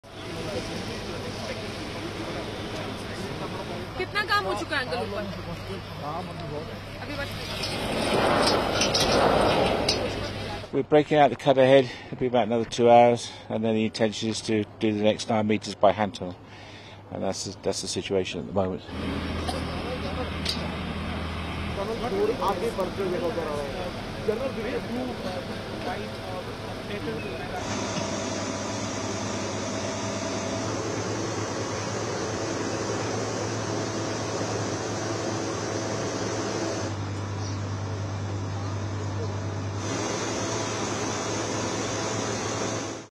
India resorts to hand tunneling to reach trapped men
Rescuers attempting to reach 41 construction workers trapped in a tunnel in the Indian Himalayas said they were resorting to manual tools and hand tunneling on Monday (November 27) amid fears of impending rain.

Video: Reuters